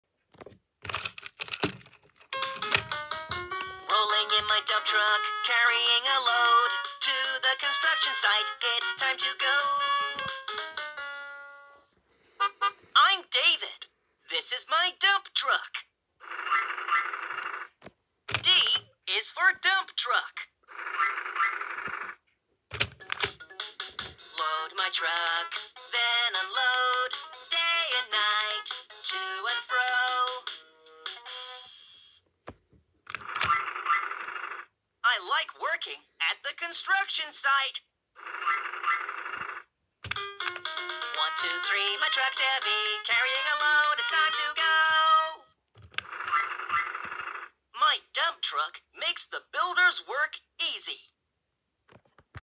VTech 2014 Go! Go! Smart Wheels Orange Dump Truck with Person
I got it from the Flea market today
I really like that one